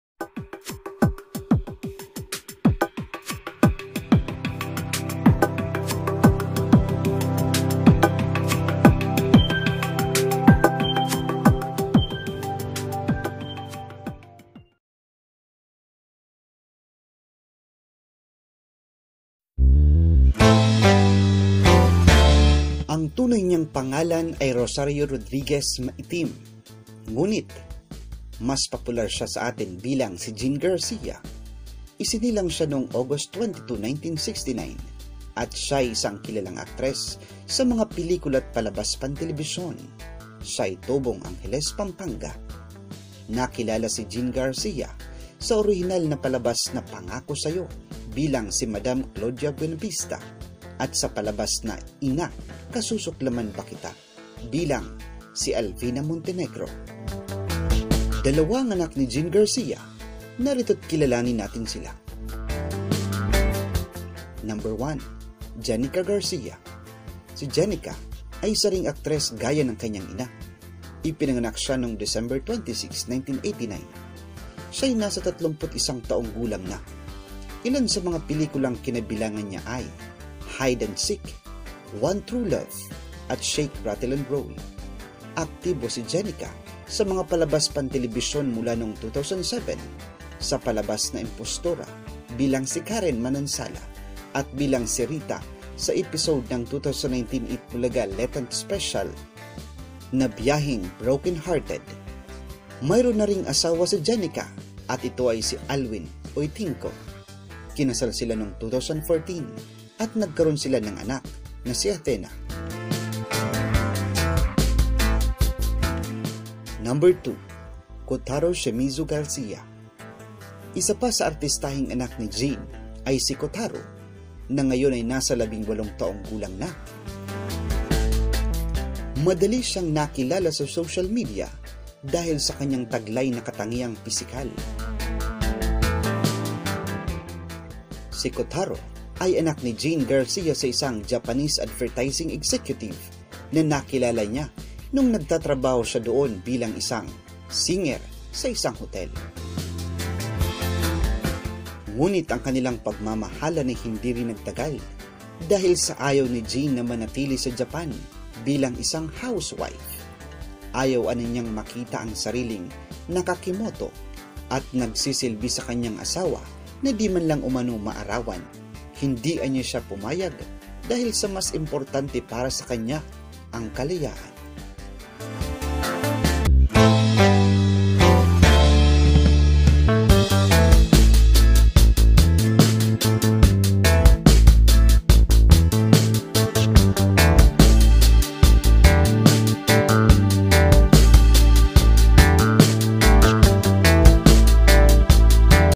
KILALANIN ANG MGA ANAK NI JEAN GARCIA
Ang tunay niyang pangalan ay si Rosario Rodriguez Maitim ngunit mas popular siya sa atin bilang si Jean Garcia. Isinilang siya noong August 22, 1969 at siya ay isang kilalang aktres sa mga pelikula at palabas pantelebisyon. Siya ay tubong Angeles, Pampanga.
Nakilala si Jean Garcia sa orihinal na palabas na Pangako Sayo bilang si Madam Claudia Buenavista at sa palabas na Ina, Kasusuklaman Ba Kita? Bilang si Alvina Montenegro.
Dalawa ang anak ni Jean Garcia. Narito’t kilalanin natin sila.
1. Jennica Garcia
 Si Jennica ay isa ring aktres gaya ng kanyan ina. Ipinanganak siya noong December 26, 1989. Siya ay nasa 31 taong gulang na. Ilan sa sa mga pelikulang kinabilangan niya ay Hide and Seek, One True Love at Shake Rattle and Roll. Aktibo si Jennica sa mga palabas pantelebisyon mula noong 2007 sa palabas na Impostora bilang si Karen Manansala at bilang si Rita sa episode ng 2019 Eat Bulaga Lenten Special na Byaheng Broken Hearted. Mayroon na ring asawa si Jennica at ito ay si Alwyn Uytingco. Kinasal sila noong 2014 at nagkaroon sila ng anak na si Athena.

2. Kotaro Shimizu  Garcia
Isa pa sa artistahing anak ni Jean ay si Kotaro na ngayon ay nasa 18 taong gulang na. Madali siyang nakilala sa social media dahil sa kanyang taglay na katangiang pisikal. Si Kotaro ay anak ni Jean Garcia sa isang Japanese advertising executive na nakilala niya noong nagtatrabaho siya doon bilang isang singer sa isang hotel. Ngunit ang pagmamahalan nila ay hindi rin nagtagal dahil sa ayaw ni Jean na manatili sa Japan bilang isang Housewife. Ayaw anay niyang makita ang sariling naka kimoto at nagsisilbi lang sa kanyang asawa at di man lang umano maarawa. Hindi anya siya pumayag dahil sa mas importante para sa kanya ang Kalayaan.